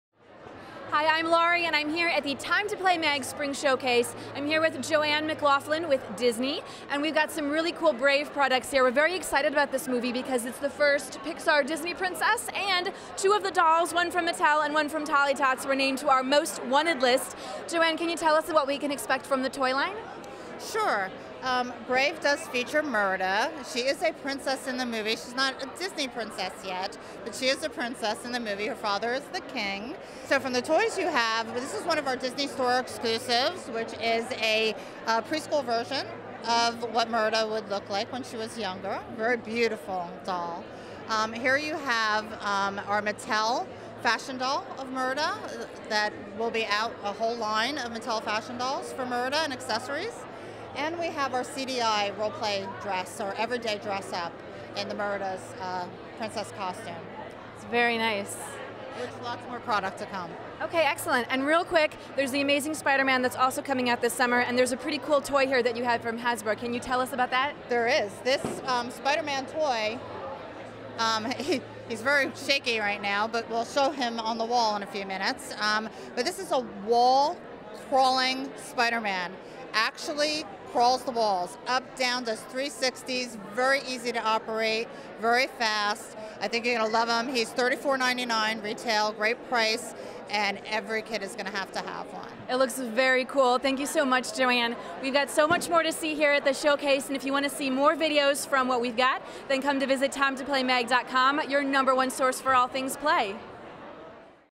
The Amazing Spiderman and Brave from Disney - 2012 Spring Showcase
We spoke to Disney about their new products at TimetoPlay's 2012 Spring & Summer showcase.